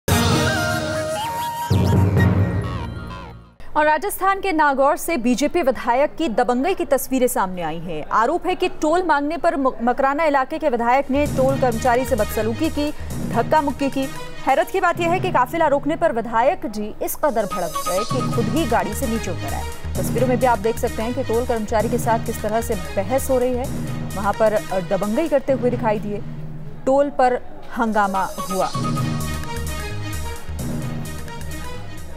Rajasthan BJP MLA creates ruckus at toll plaza
A Rajasthan BJP MLA's blatant abuse of power was on full display as he brazenly refused to pay toll on the Jaipur-Nagaur Highway.